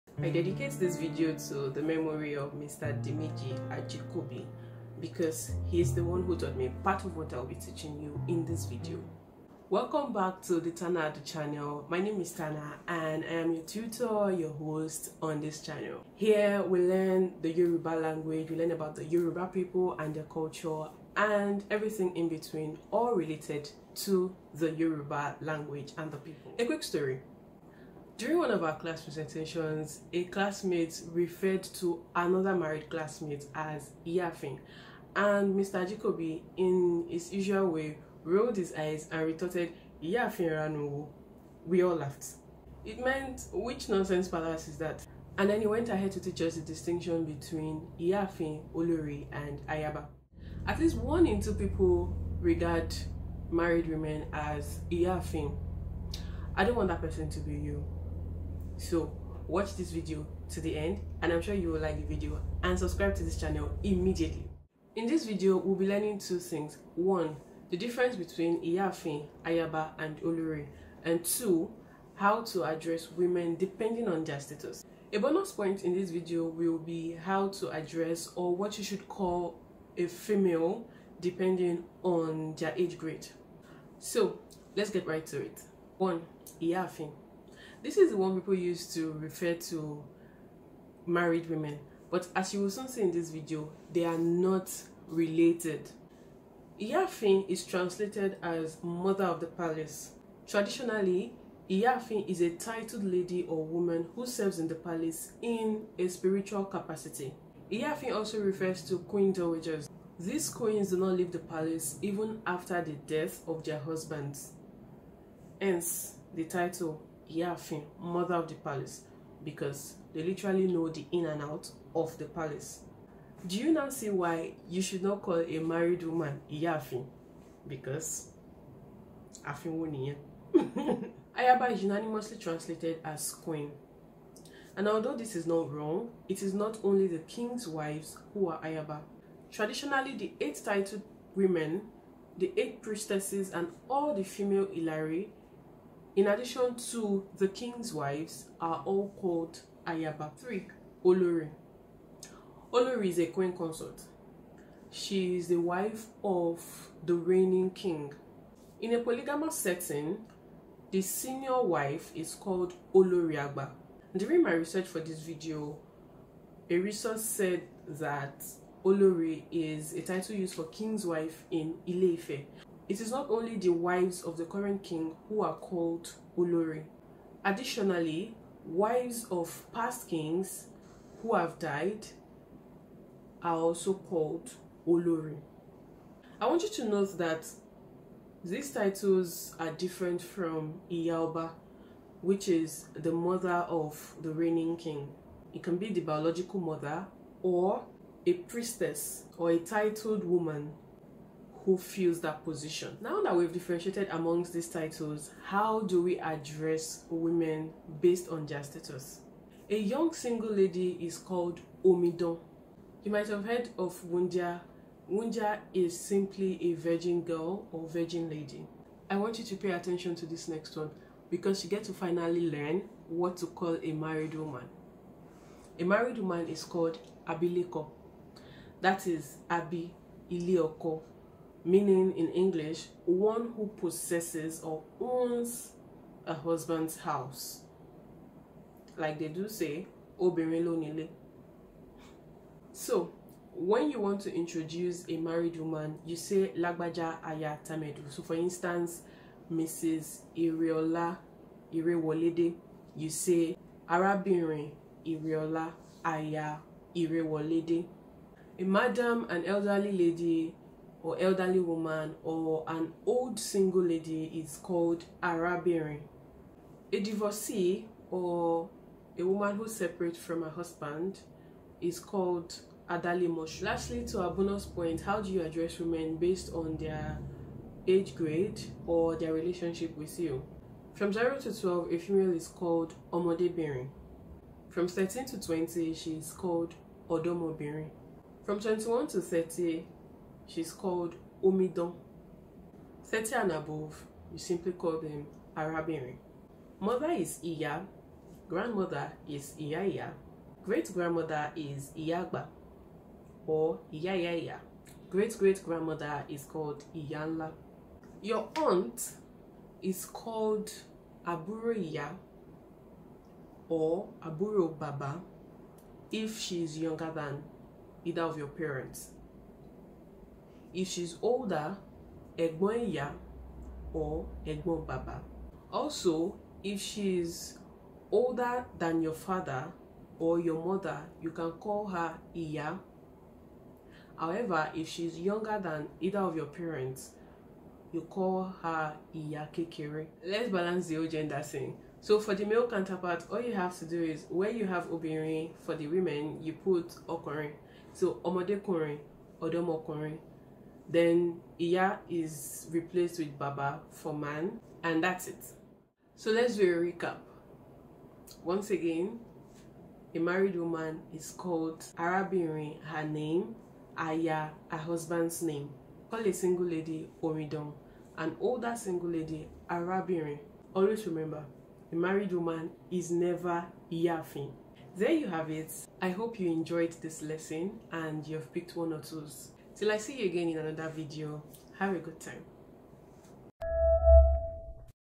Diiferent ways to say "Queen" in Yoruba Language | How to say "Mrs" in Yoruba
- the difference between "Iya aafin, olori and ayaba".
- what to call a married woman (Mrs X) in Yoruba
- nomenclatures for females based on age-grade
- nomenclatures for the women in your life/family